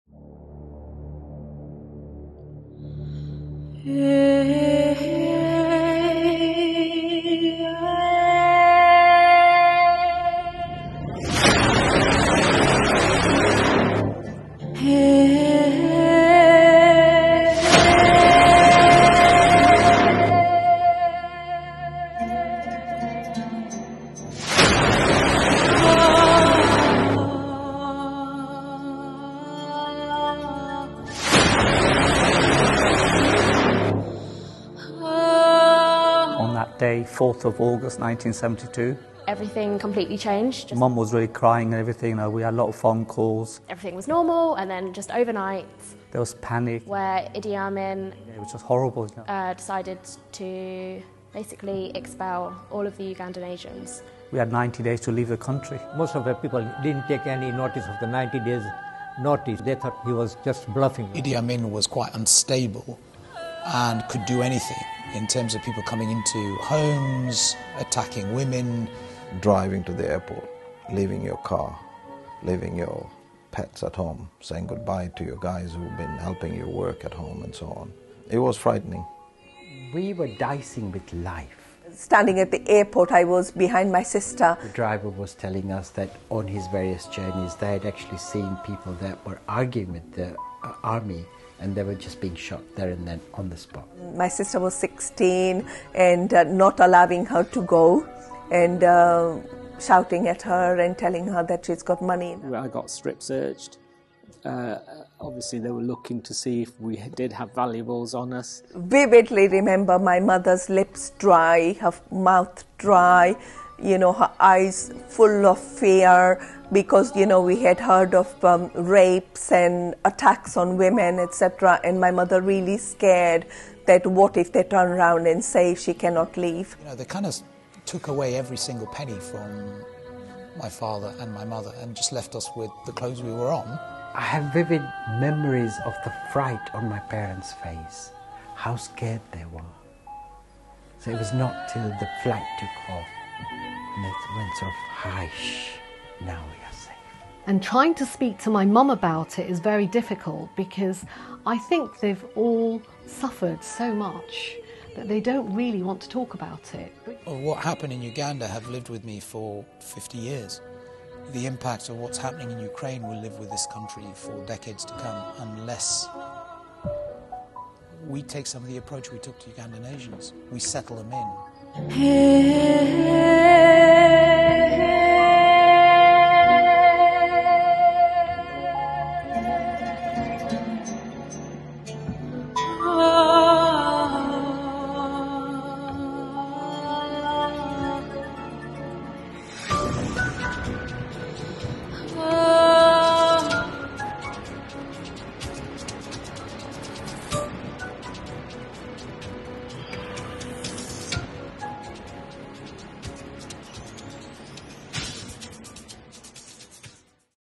4th August 1972 - Idi Amin edict on Uganda Asians expulsion.
It is the 50th Anniversary of the Ugandan Asian expulsion when Idi Amin announced that Asians need to leave the country within 90 days.